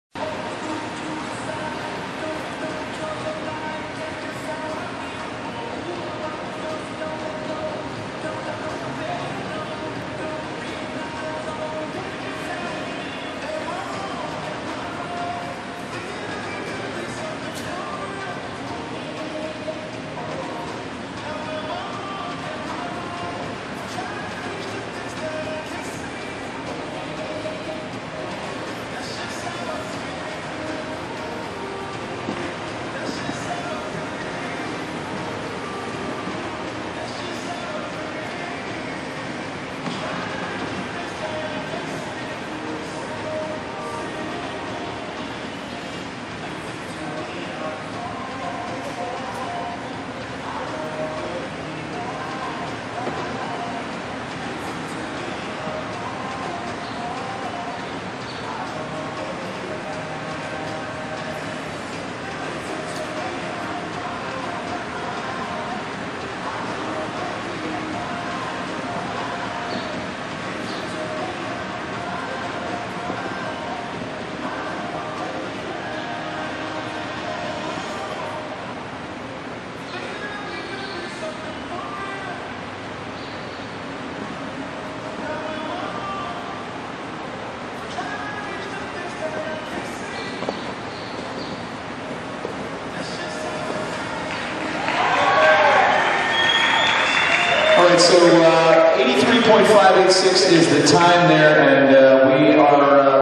Video of ADRIANE BB ridden by JUAN MANUEL BOLANOS from ShowNet!
ADRIANE BB ridden by JUAN MANUEL BOLANOS at Kentucky Summer Classic 2014.  This round is from class 432, OPEN JUMPER 1.35m II 2b and finished 8 of 30.